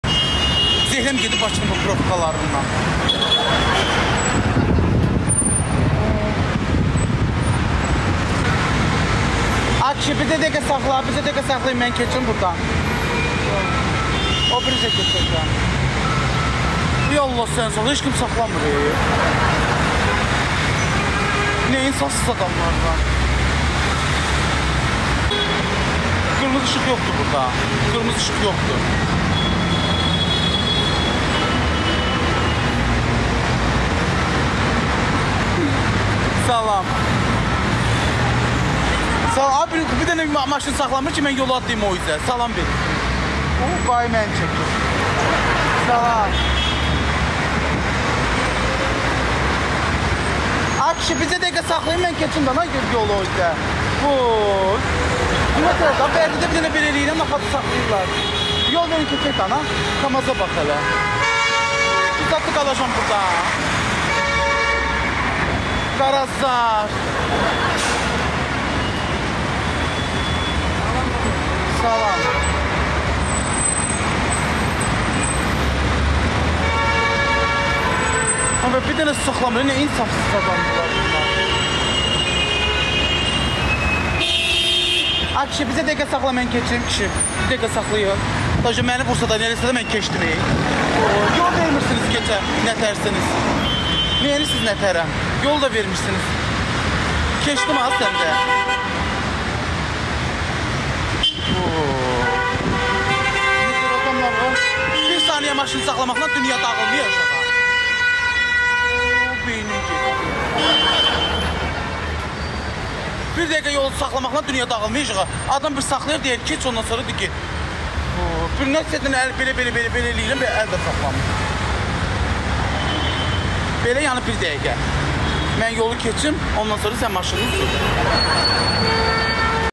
Baboş: "Ay Kişi, Saxla Keçim Dana! Boooy!" | Baboş TikTok
TikTok platformasında Azərbaycanın N1 fenomeni olan Baboş ləqəbli TikToker Cabbarov Babək, bugün Bakı şəhərində yolu keçərkən xeyli çətinlikə qarşılaşır. Hətta, bir sürücüyə səslənərək: "Ay kişi saxla keçim dana boooy" deyir. Baboşun yolu keçərkən yaşadığı bu anlar canlı yayımdakı izləyicilərin xeyli gülməsinə səbəb olur.

Videonu bəyəndinizsə və daha çox Baboşun gülməli və maraqlı videolarını izləmək istəyirsinizsə, Baboş Tv kanalımıza abunə olmağı unutmayın! Hər birinizə xoş izləmələr!